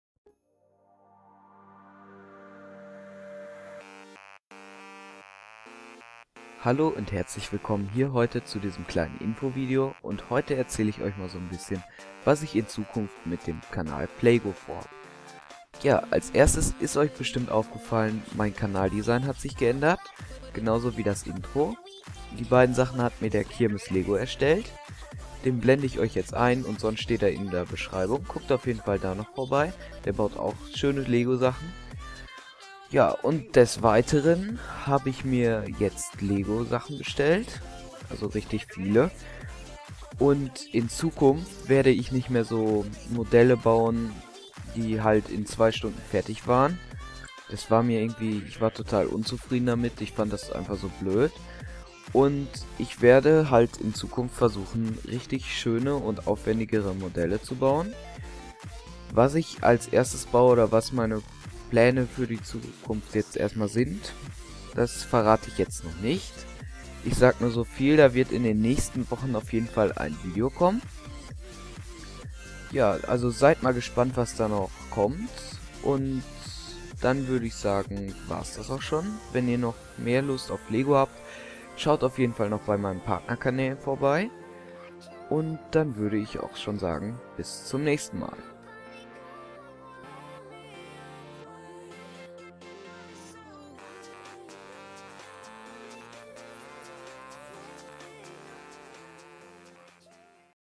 Info PlayGo 2.0?
Ein kurzes Informations Video über die Zukunft von PlayGo